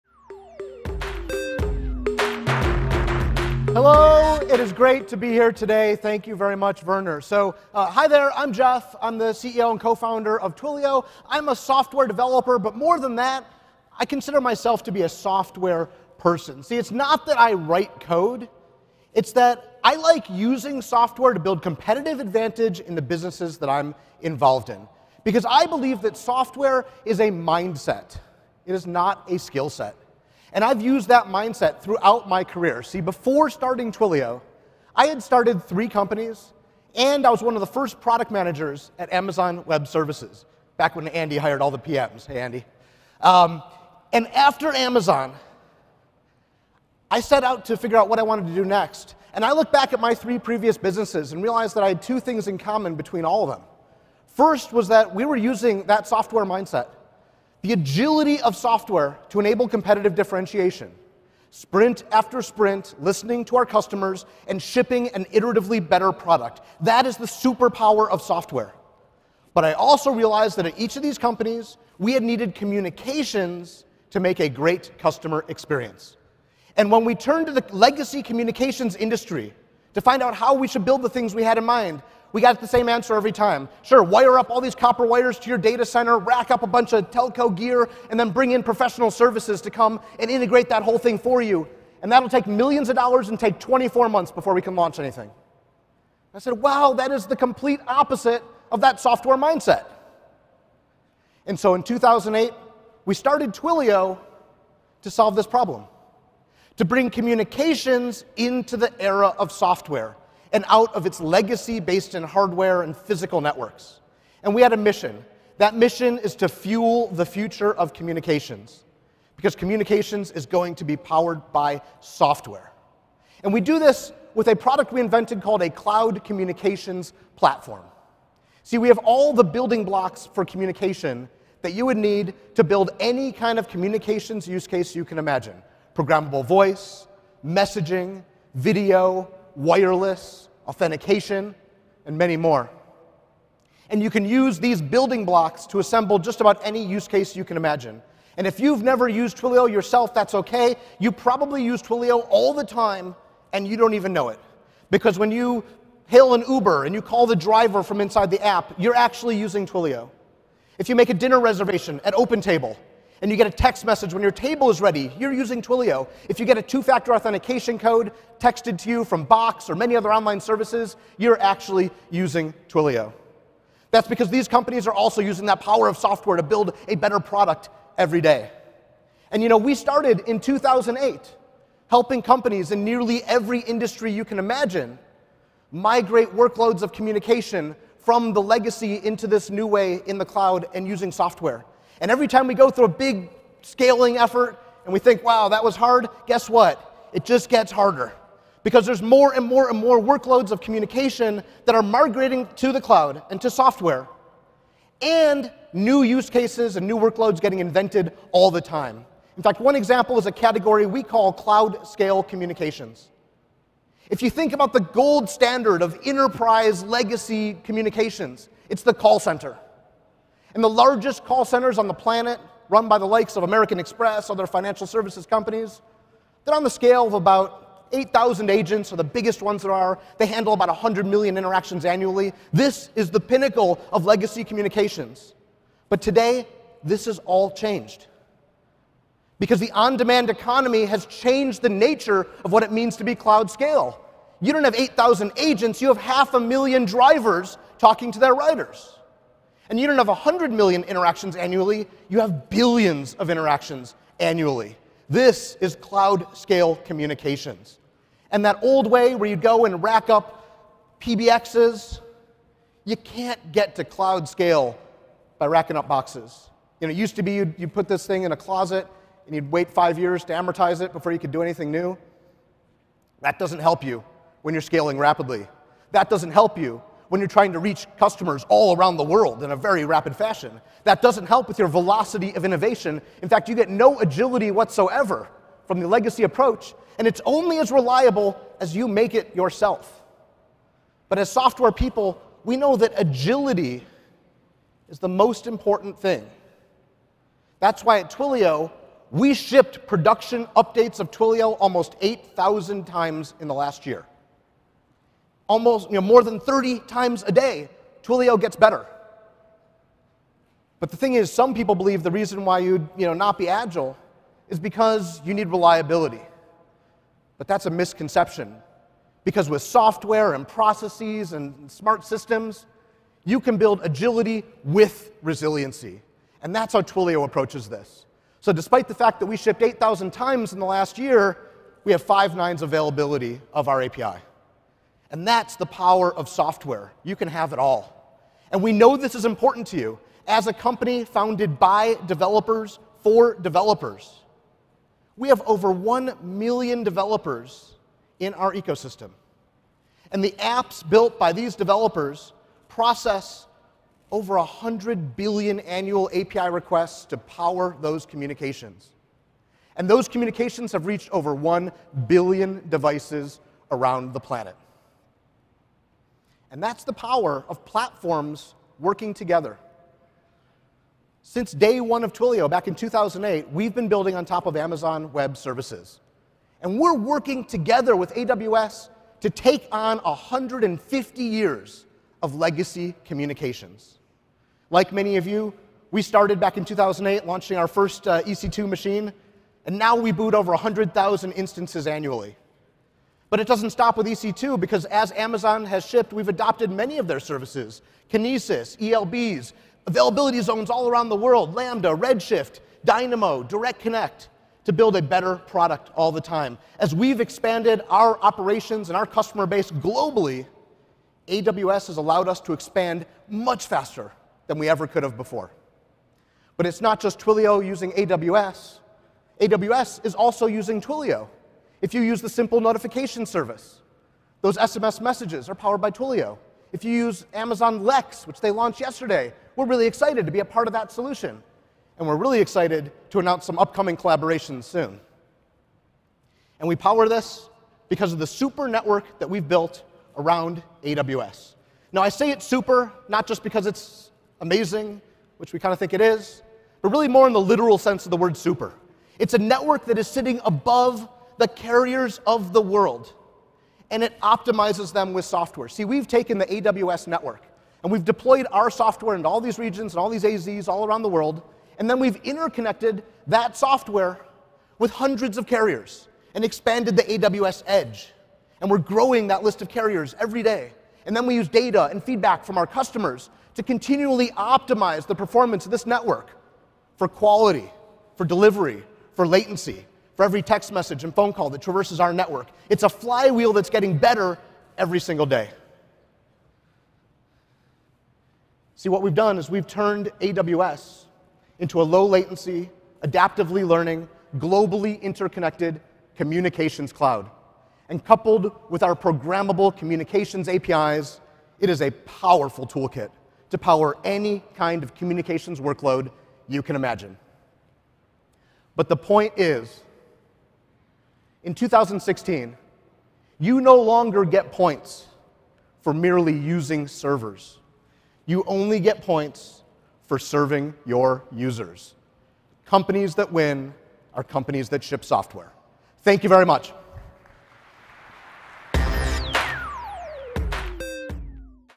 Jeff Lawson Talks About How AWS Powers Twilio
To learn more about how AWS can help host your web and mobile apps in the cloud, visit our websites and website hosting details

Twilio uses AWS to deploy an average of 30 features a day and maintain 99.999% availability. Twilio provides a communications platform as a service to customers like Uber, Netflix, and Airbnb. Twilio is all in on AWS to power it’s cloud-scale communication apps globally. Cofounder, CEO, and Chairman Jeff Lawson spoke onstage at re:Invent 2016.